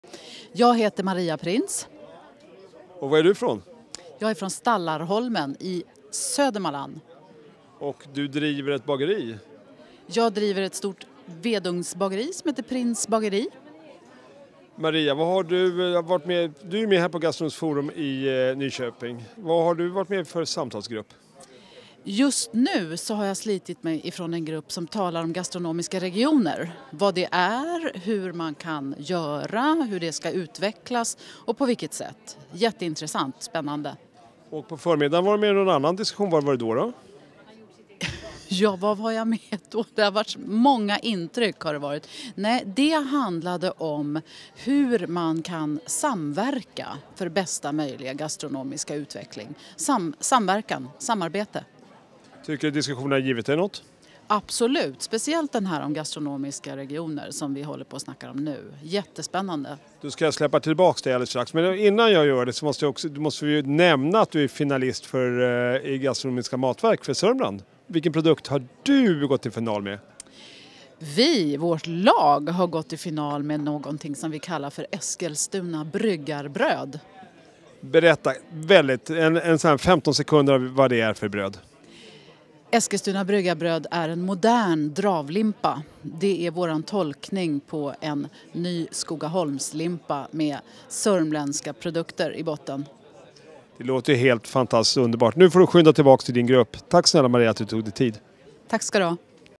Maria Printz - Printz Bageri
Printz Bageri är ett vedugnsbageri i Stallarholmen, Sörmland. Maria Printz är finalist i Matverk 2013 och engagerad deltagare i Gastronomiska Samtal i Nyköping.

Aktiviteter

Gastronomiskt Forum 2013 - under ett par dagar samlas några hundratal personer med framträdande roller och uppgifter inom den svenska matbranschen för en öppen dialog under fria kreativa former kring ett givet tema. En unik möjlighet för alla inbjudna att både vara med och driva utvecklingen av det svenska köket och hämta inspiration för den egna verksamheten.

Gastronomiska Matverk 2013 - är en produktutvecklingstävling som skapar moderna livsmedelsprodukter med rötter i lokala traditioner och råvaror. Bakom bidragen finns lag med råvaruproducent, matlagare och förädlare från alla Sveriges landskap. 210 nya matprodukter av 780 produktutvecklare kommer att se dagens ljus. Tre riksvinnare koras på Gastronomiskt Forum.